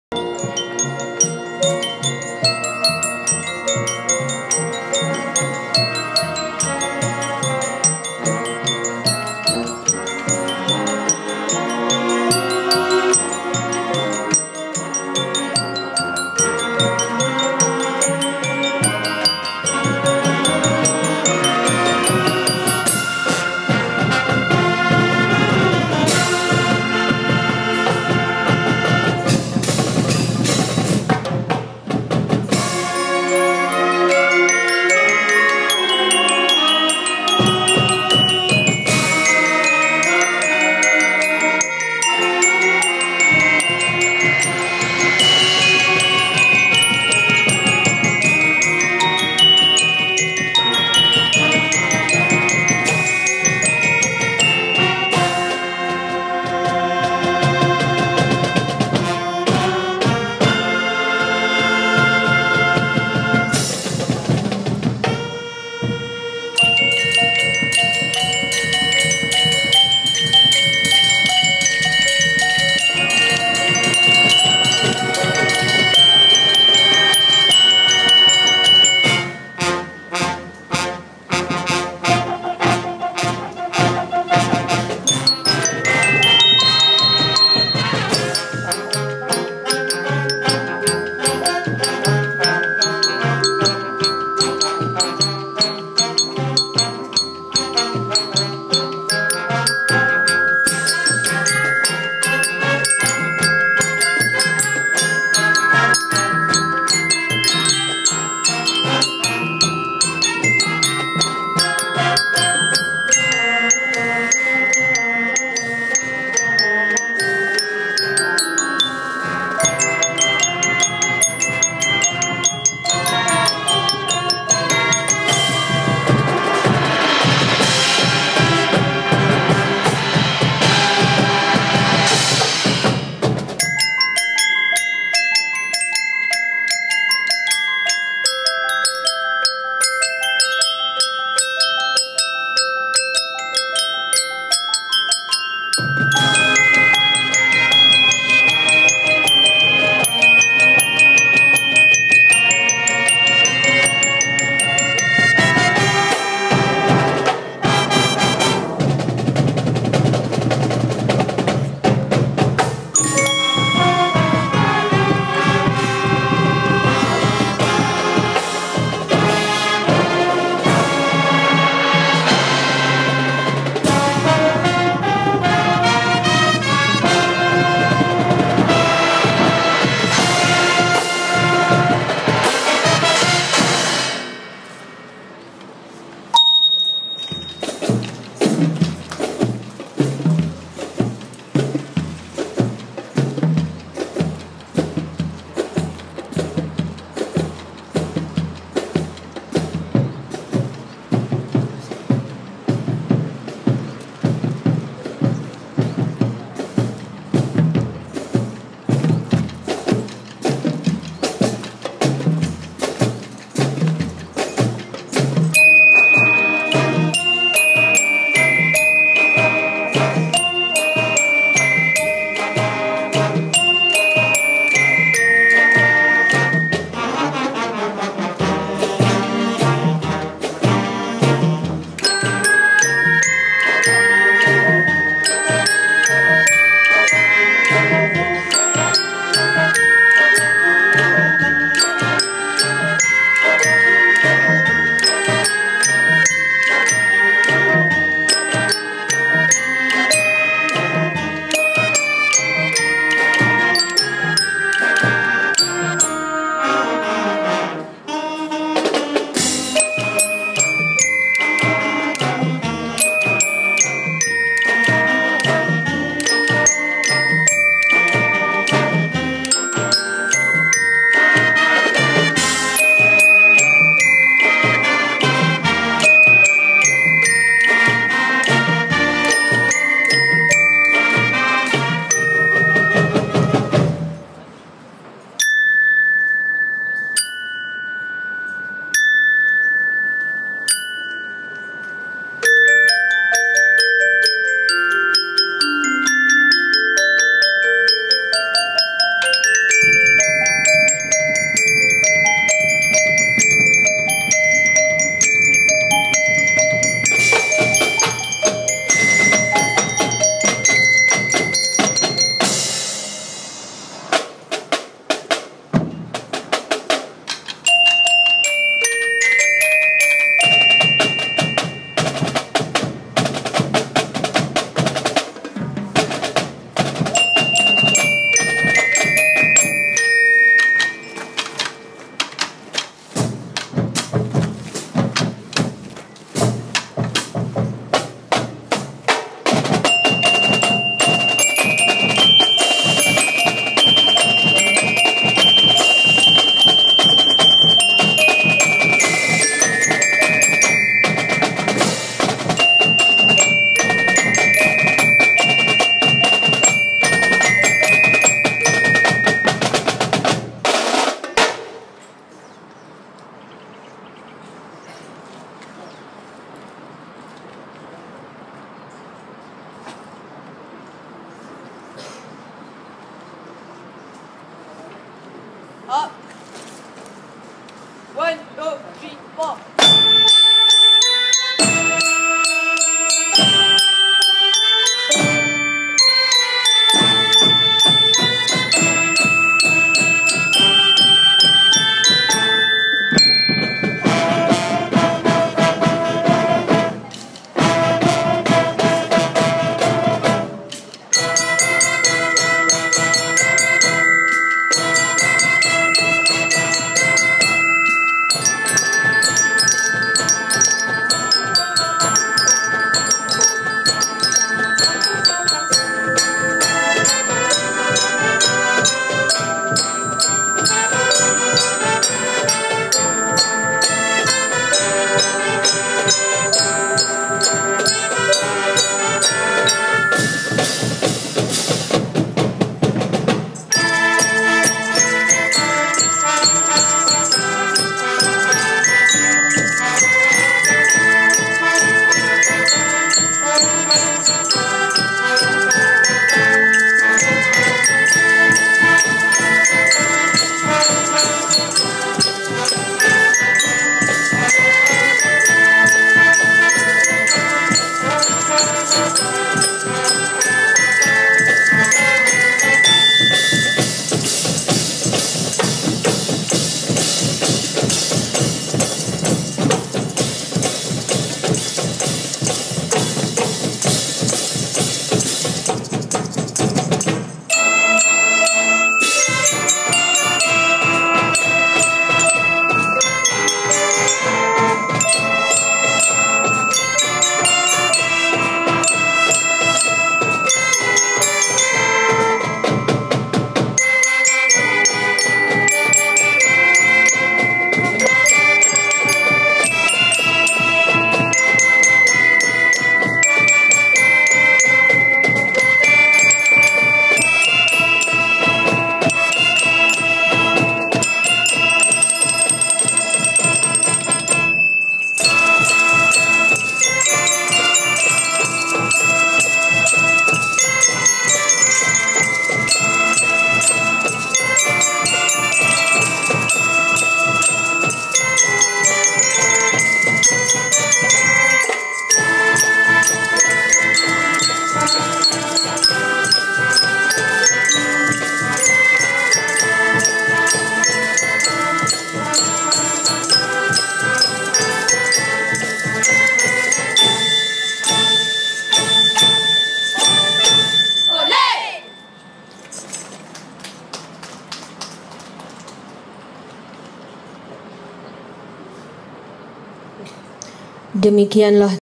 STF Band - KRS 2015 Closer Performance
Event: Perkhemahan dan Pentauliahan Kadet Remaja Sekolah (KRS) Sekolah Berasrama Penuh Peringkat Kebangsaan Ke-7, 2015

Venue: Pusat Kokurikulum Johor, Kem Tasik Utara, Jabatan Pendidikan Johor, JB

Slowly but surely posting old videos stored in the crypt :)